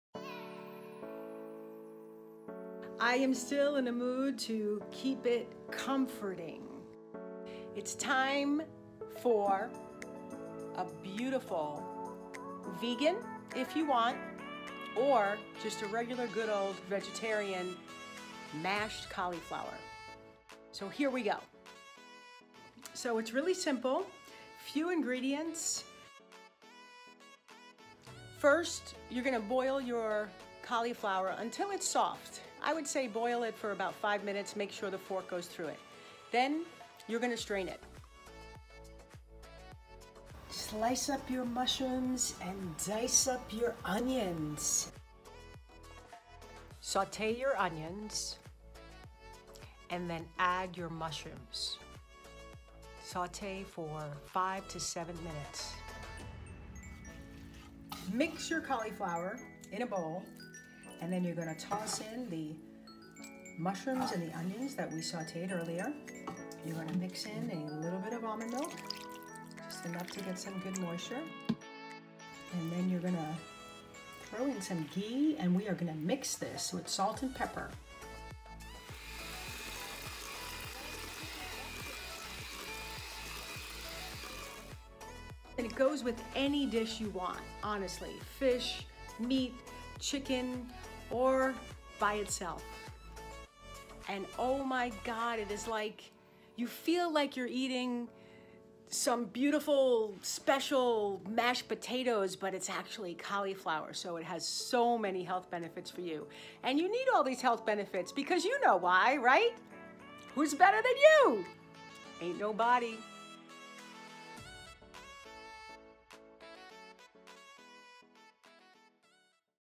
Dish With Trish: Mashed Cauliflower With Mushrooms
I’m still in the mood to keep it comforting with this vegan or vegetarian dish.  This tastes like you’re eating a special mashed potato dish, but … it’s cauliflower! So wonderful and healthy.

INGREDIENTS
1 head of cauliflower
1 onion, diced
2 cups of mushrooms, sliced
3 Tablespoons of ghee, or butter
½ cup of almond milk

Boil cauliflower until soft, approximately 5 minutes until a fork goes through.
Strain this and put cauliflower in a big bowl.
Saute onions in olive oil.
Add mushrooms, and cook for 5-7 minutes.
Add your onions and mushrooms to your bowl of cauliflower.
Mix in almond milk, ghee/butter, salt and pepper to taste.
Mix it all with a hand blender.

Who’s Betta Than You?  Nobody.

Music @iksonofficial